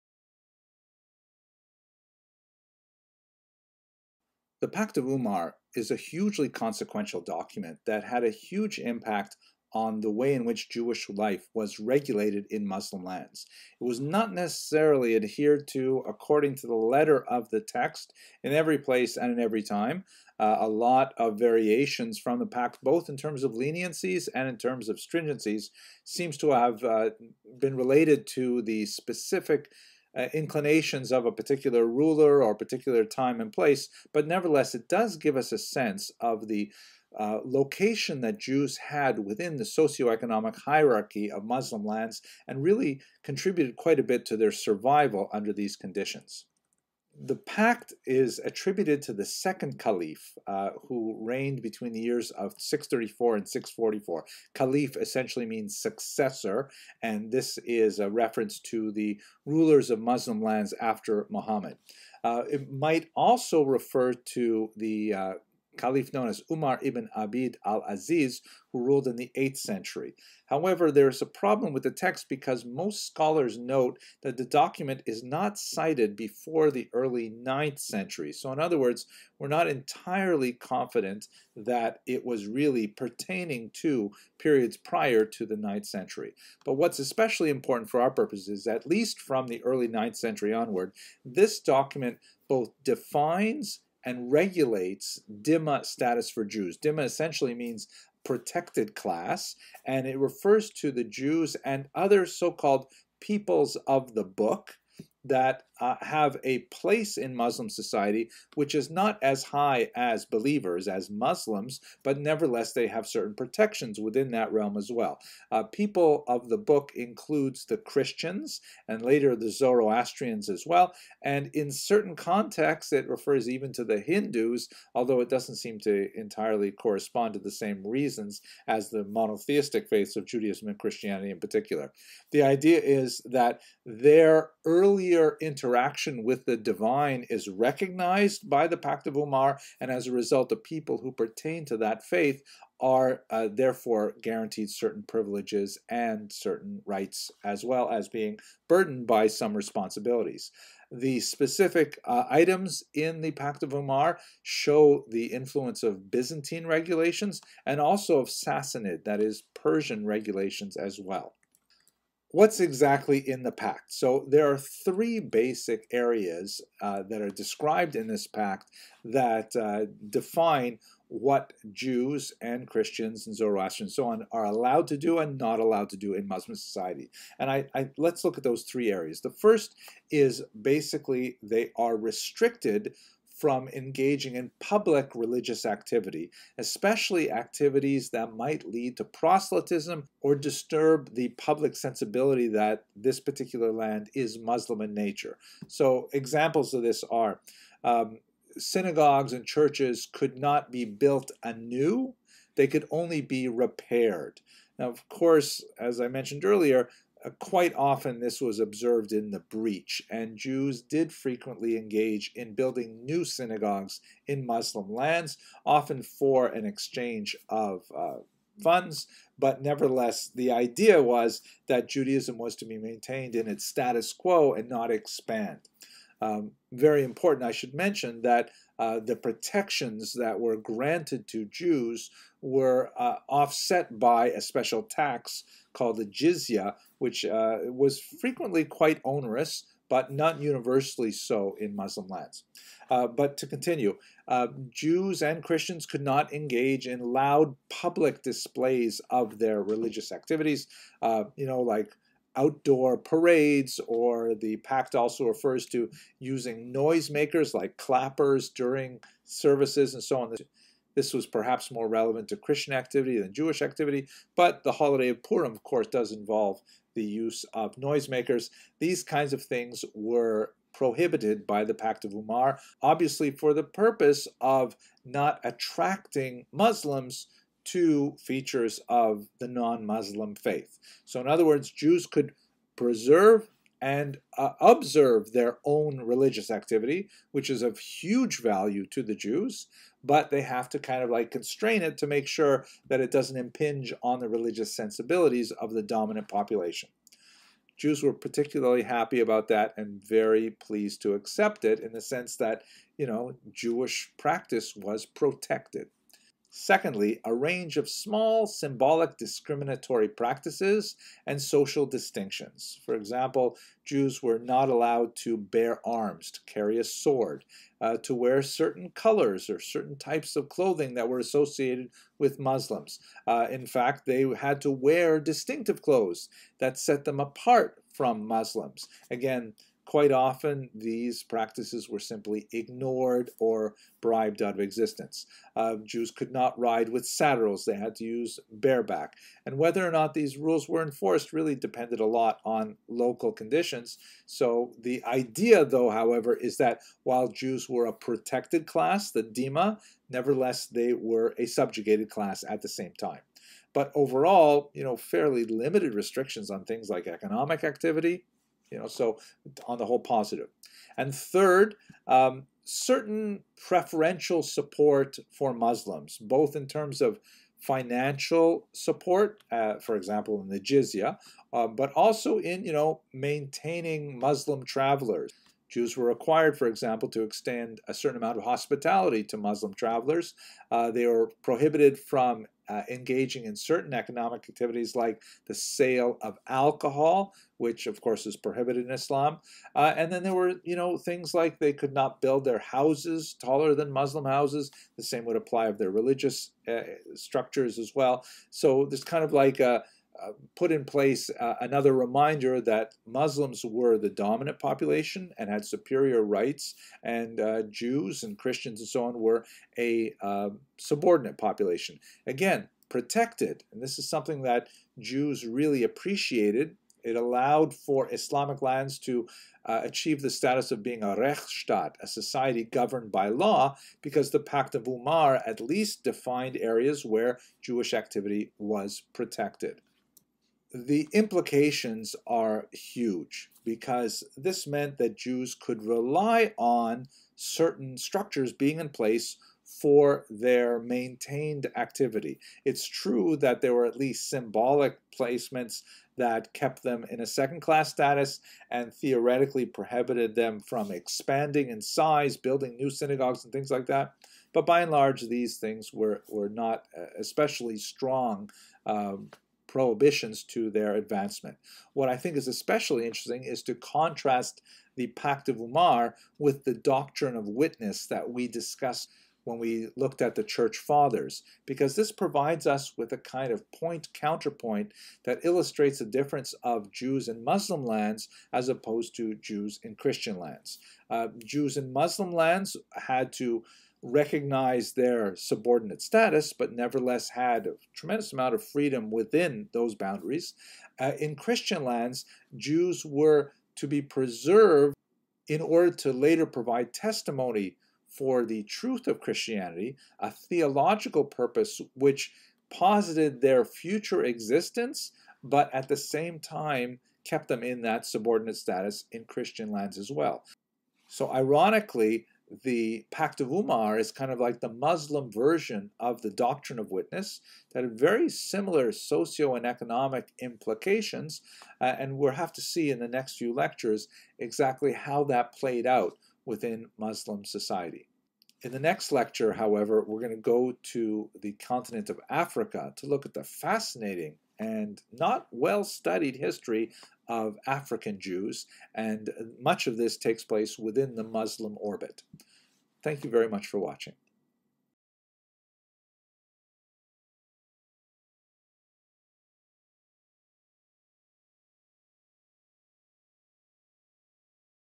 53. The Pact of Umar (Jewish History Lab)
Brief discussion of The Pact of Umar and its significance for Jewish history.